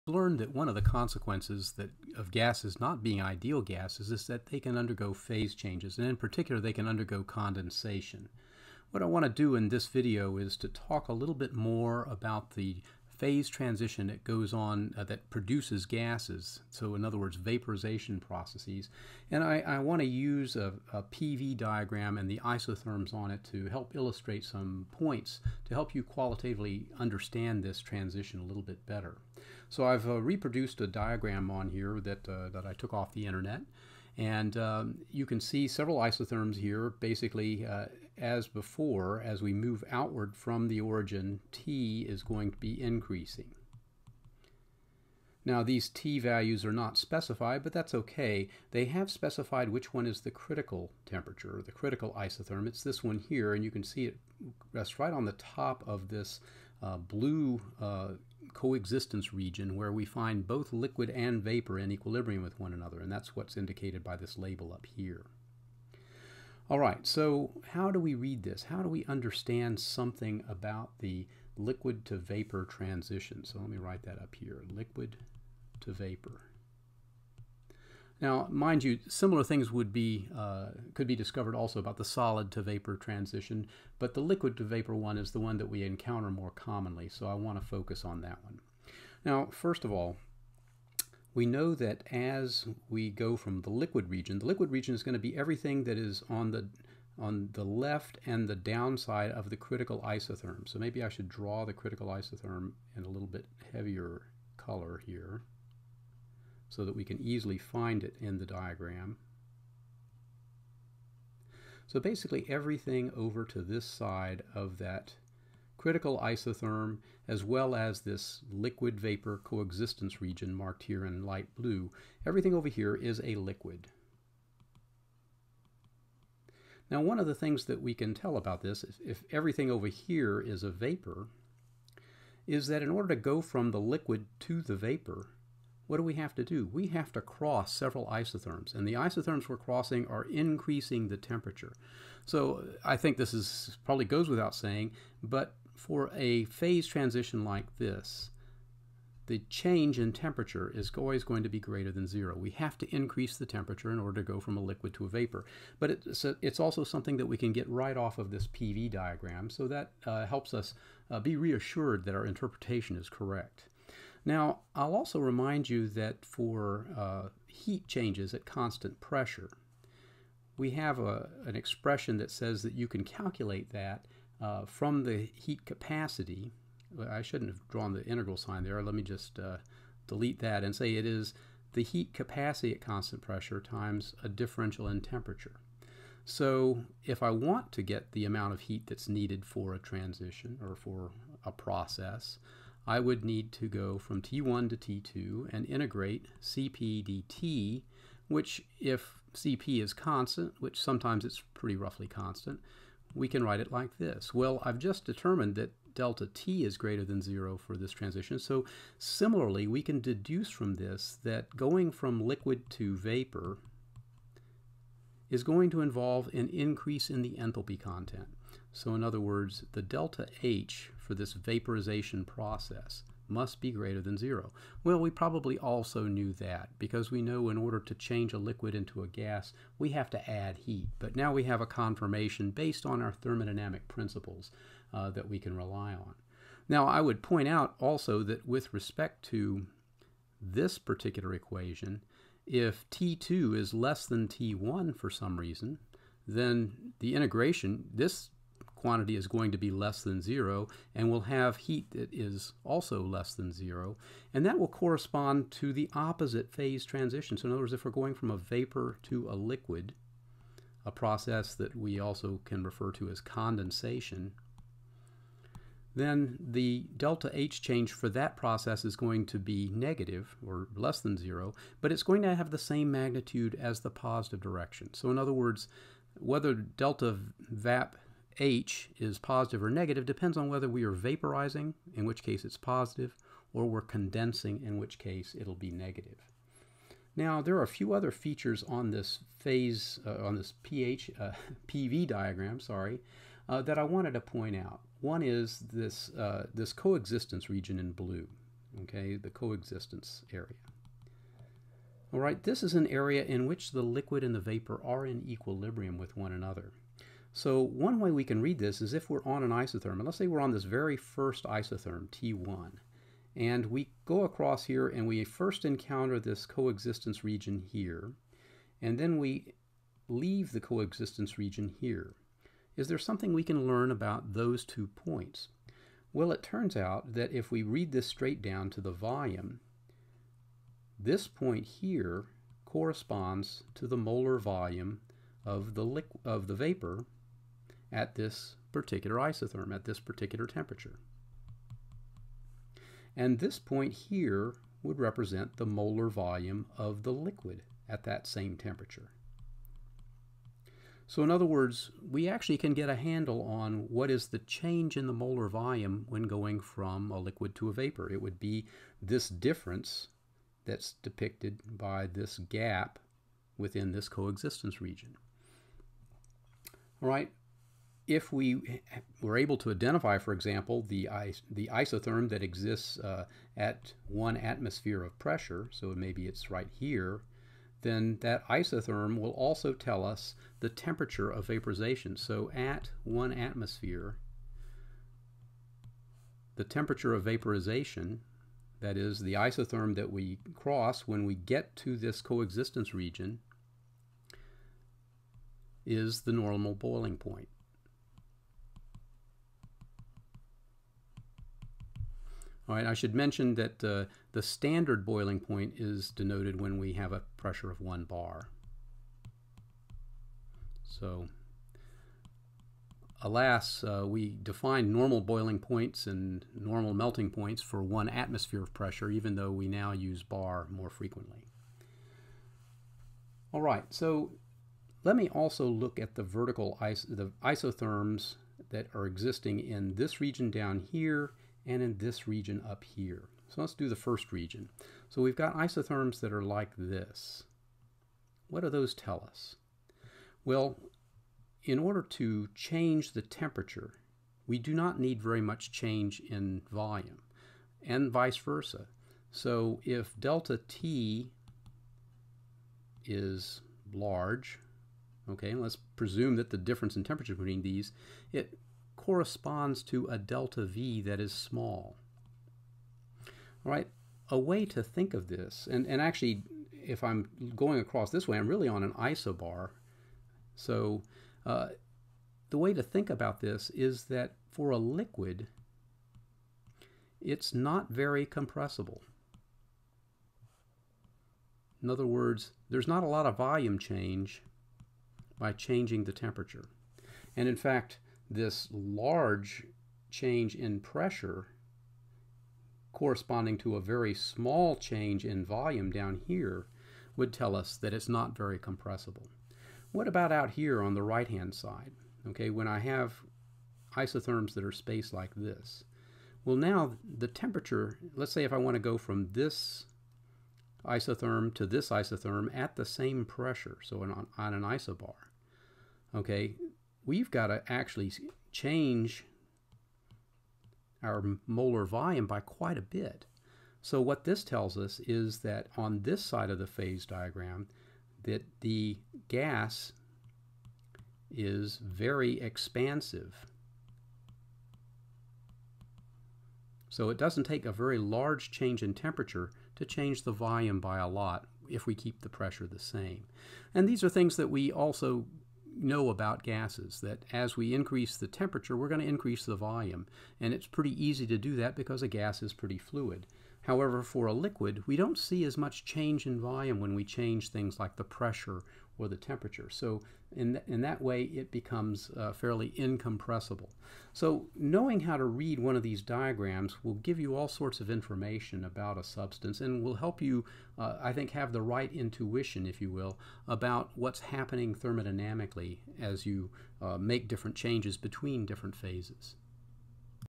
Lesson 4B PV-Diagrams and Isotherms
This video provides an overview of the information one can infer about the vaporization process from a PV-diagram showing isotherms.